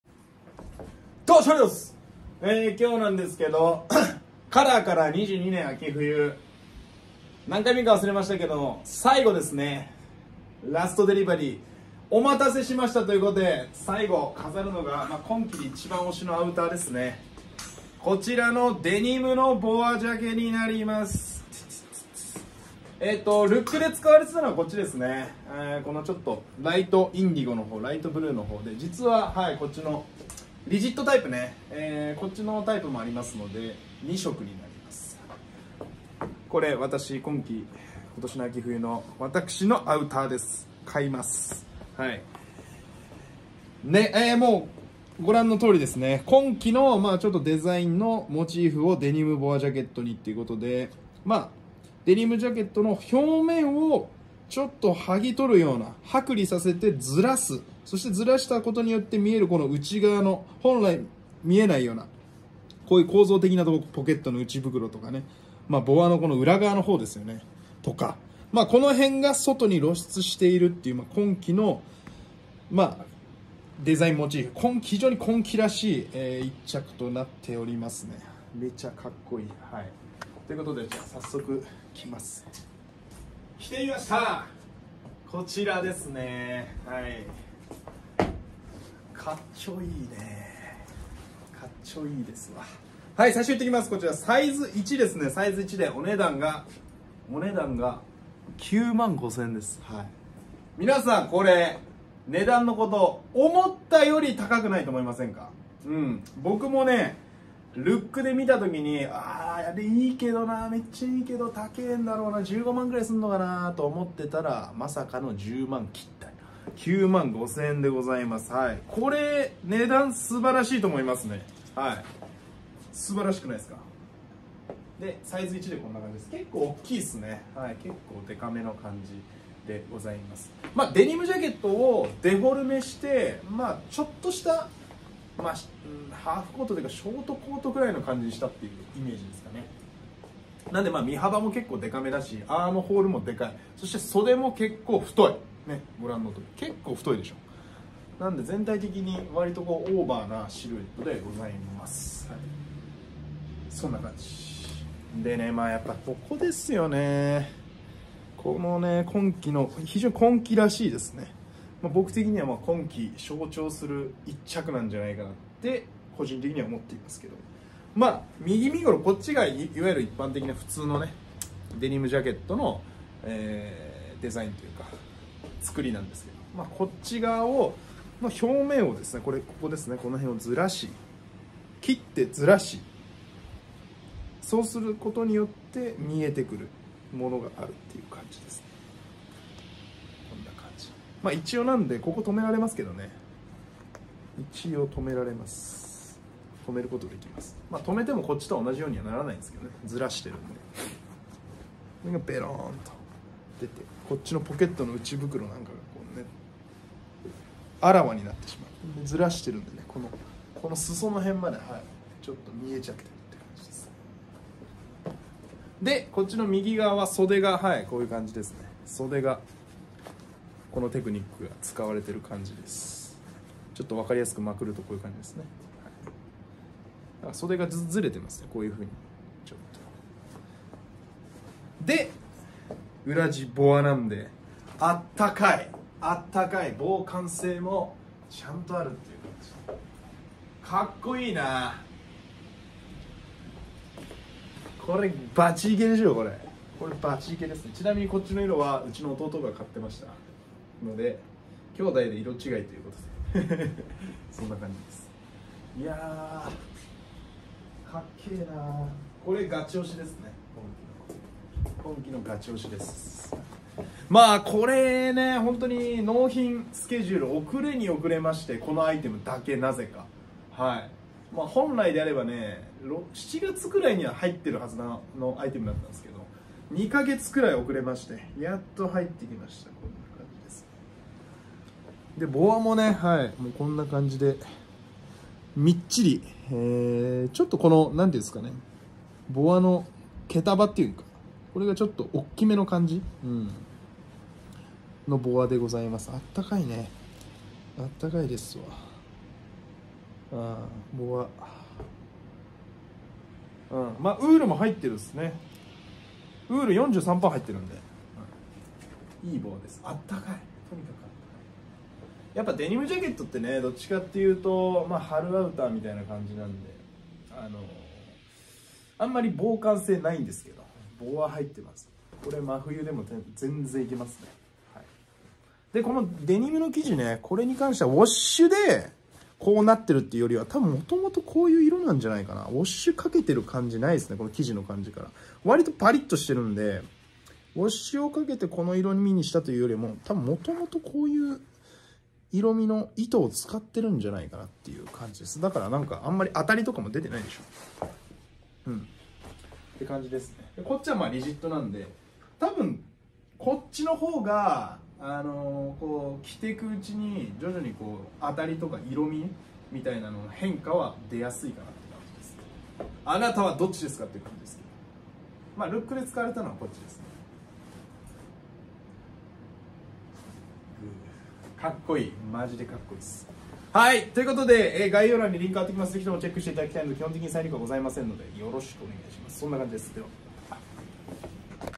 【最速】kolor 22FW 3rd！お待たせしましたデニムボアジャケット！！今季のテクニックを使った一着！！俺の秋冬アウターに決まり
詳細はこちら！※基本的に再入荷はございません。

ついでにアマゾン欲しいものリストはこちら！

ついでにブログもよろしく！